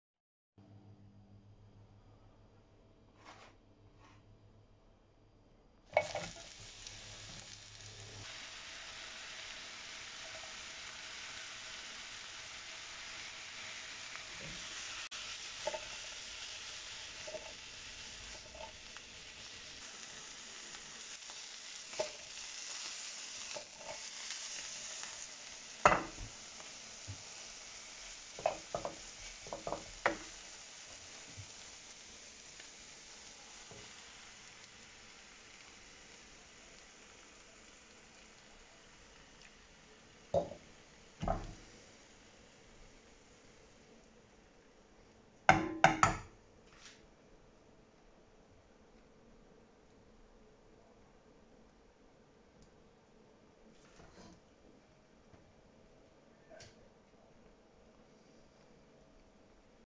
How to make Mushroom omlet with parsley(mantarli yumurta)for bodybuilding
I got a village butter here made it with mushroom parsley and 2-3 eggs after workout meal. The drink is filter coffee v60 or French press and orange juice